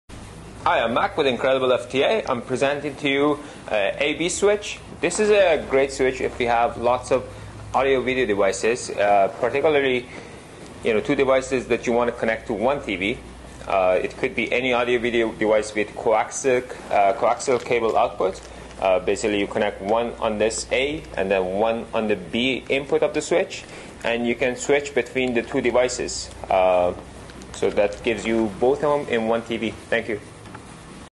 Eagle Aspen A/B Coaxial Switch
This brief video explains the A/B Switch, which allows you to toggle between 2 devices that have a coaxial output.